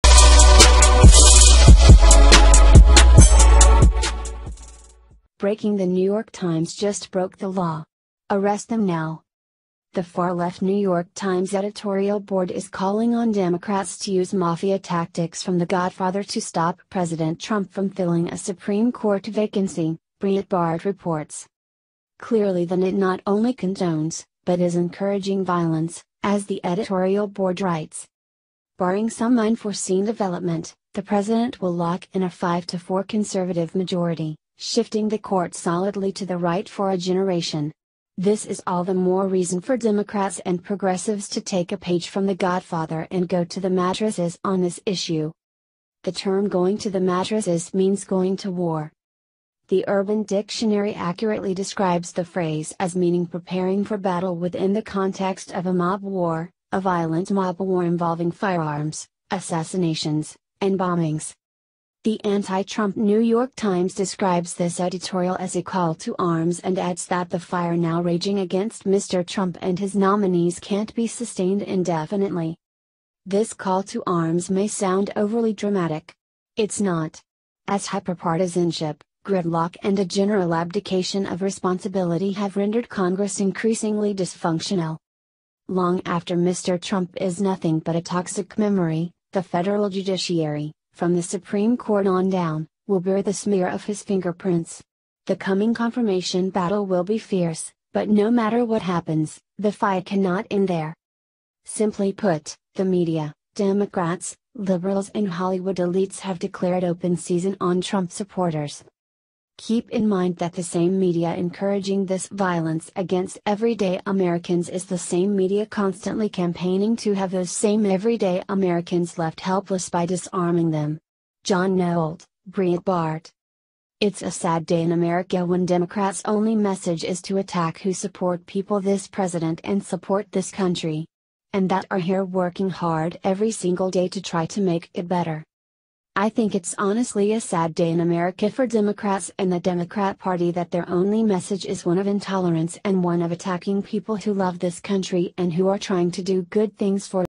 BREAKING The New York Times Just Broke THE LAW… ARREST THEM NOW!
The far-left New York Times editorial board is calling on Democrats to use mafia tactics from The Godfather to stop President Trump from filling a Supreme Court vacancy, Breitbart reports.

Clearly the NYT not only condones, but is encouraging violence, as the editorial board writes:

Barring some unforeseen development, the president will lock in a 5-to-4 conservative majority, shifting the court solidly to the right for a generation. This is all the more reason for Democrats and progressives to take a page from “The Godfather” and go to the mattresses on this issue.

The term “going to the mattresses” means going to war.

The Urban Dictionary accurately describes the phrase as meaning “preparing for battle” within the context of a mob war, a violent mob war involving firearms, assassinations, and bombings.

The anti-Trump New York Times describes this editorial as a “call to arms” and adds that the “fire now raging against Mr. Trump and his nominees can’t be sustained indefinitely.”

This call to arms may sound overly dramatic. It’s not. As hyperpartisanship, gridlock and a general abdication of responsibility have rendered Congress increasingly dysfunctional.

Long after Mr. Trump is nothing but a toxic memory, the federal judiciary — from the Supreme Court on down — will bear the smear of his fingerprints. The coming confirmation battle will be fierce, but no matter what happens, the fight cannot end there.

Simply put, the media, democrats, liberals and Hollywood elites have declared “open season” on Trump supporters.

“Keep in mind that the same media encouraging this violence against everyday Americans is the same media constantly campaigning to have those same everyday Americans left helpless by disarming them.” ~ John Nolte – Breitbart

“It’s a sad day in America when Democrats only message is to attack who support people this president and support this country. And that are here working hard every single day to try to make it better.

I think it’s honestly a sad day in America for Democrats and the Democrat Party that their only message is one of intolerance and one of attacking people who love this country and who are trying to do good things for it.” ~ White House press secretary Sarah Huckabee Sanders